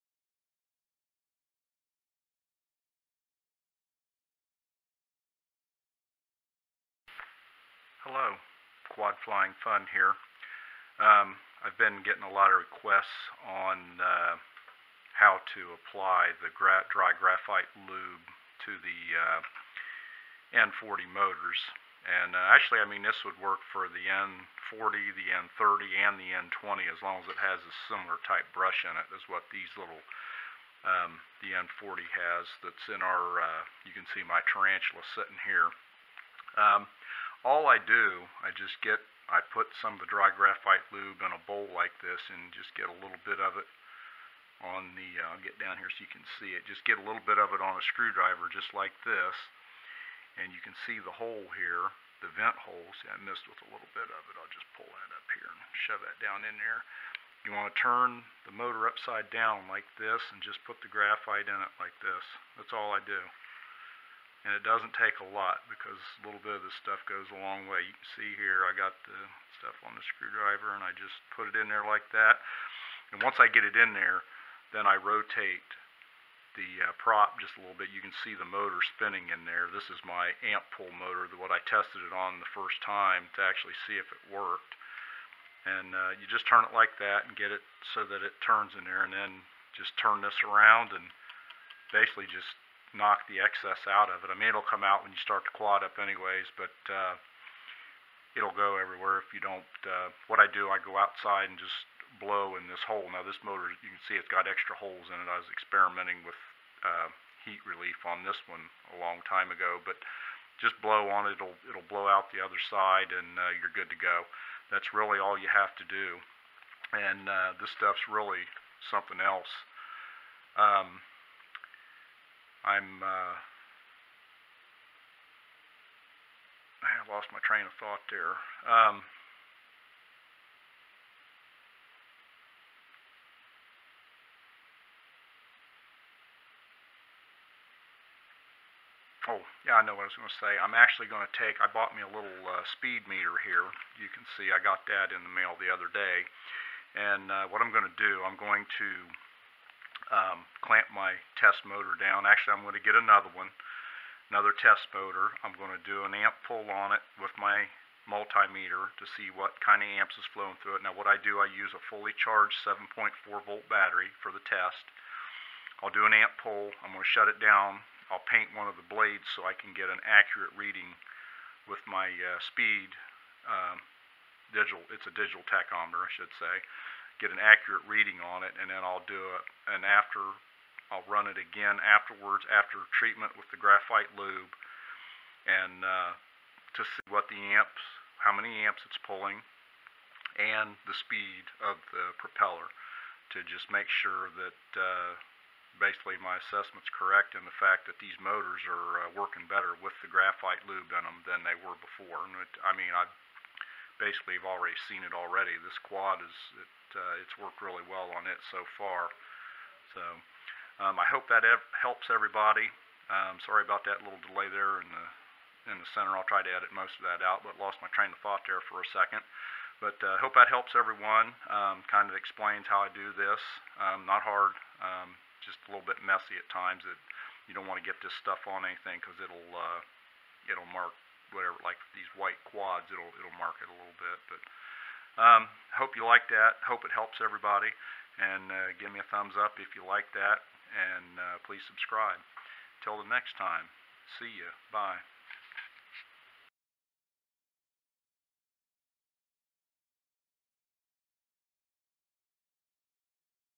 Drone applying dry graphite lube to the N40 DC motor
This video details the procedure used to apply dry graphite lube to the N40 motors used in the Yi Zhan Tarantula X6.